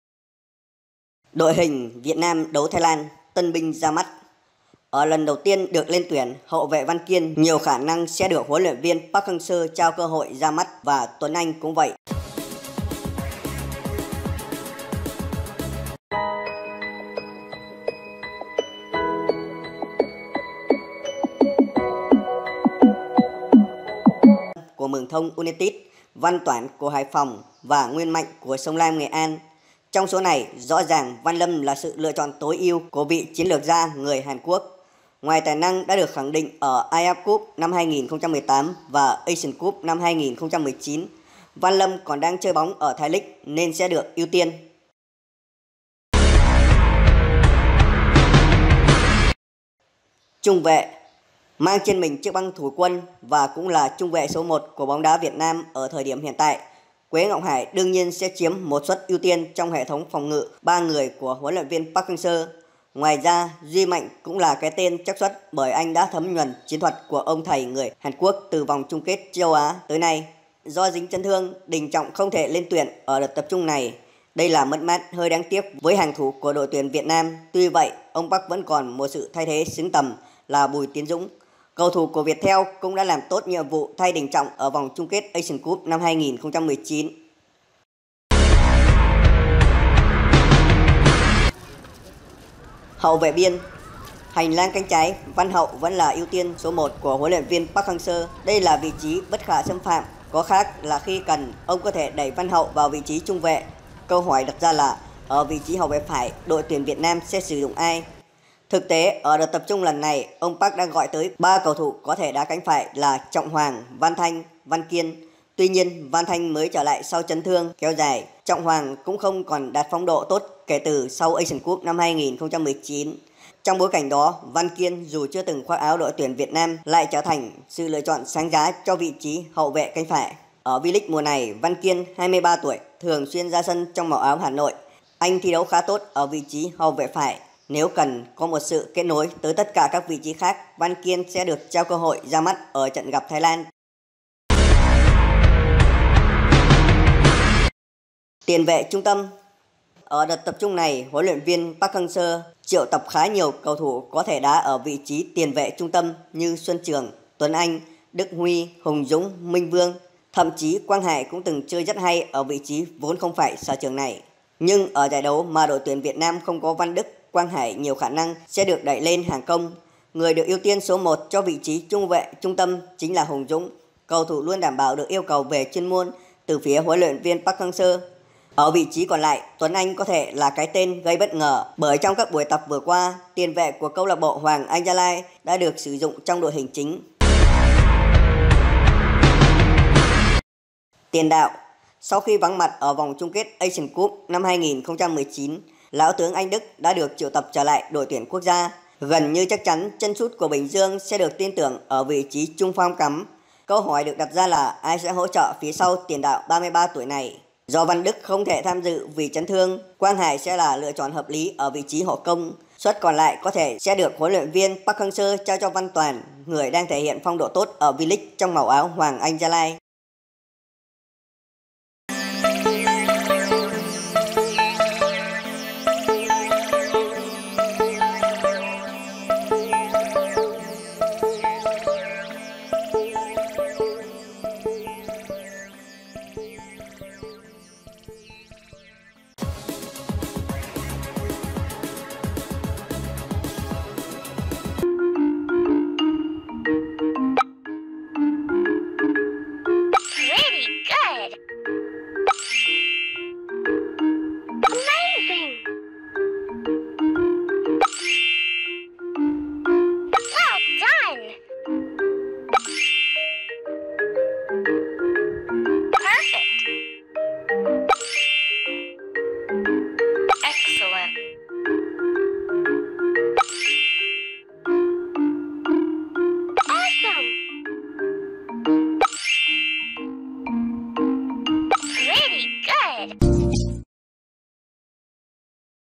Trực tiếp bóng đá Việt nam Thái lan- bình luận trước trận đấu-đội hình mạnh nhất?
Nếu được HLV Sirisak Yodyardthai cho ra sân đấu ĐT Việt Nam hôm nay, tiền đạo đội trưởng Teerasil Dangda sẽ có tròn 100 trận đấu khoác áo "Bầy voi chiến". Trong 99 trận đã từng chơi cho tuyển Thái Lan trước đây, chân sút đang thuộc biên chế Muanthong United (đồng đội với thủ thành Đặng Văn Lâm) đã có 43 bàn thắng, trong đó có 2 lần anh sút tung lưới ĐT Việt Nam đều vào năm 2008 ở cúp giao hữu T& T Cup và giải Vô địch Đông Nam Á (AFF Cup).